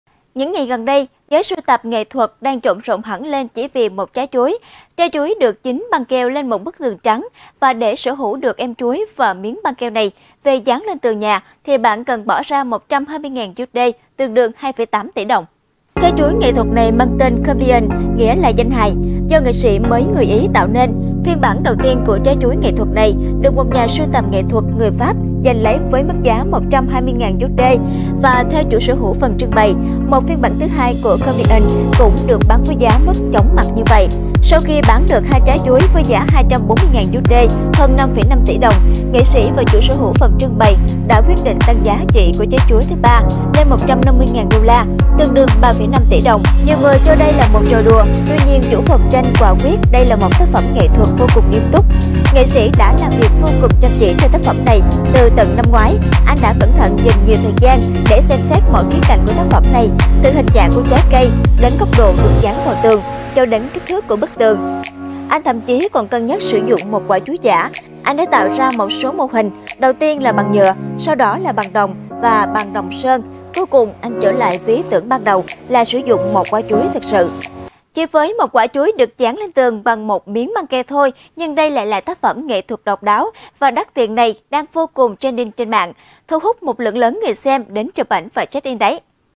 Quả chuối dán tường bằng băng keo trị giá 120.000 USD đang gây 'bão'
. Những ngày gần đây, giới sưu tầm nghệ thuật đang chộn rộn hẳn lên chỉ vì một.. trái chuối. Trái chuối được dính băng keo lên một bức tường trắng, và để sở hữu được em chuối và miếng băng keo này về dán lên tường nhà, bạn cần bỏ ra 120.000 USD, tương đương 2,8 tỷ đồng.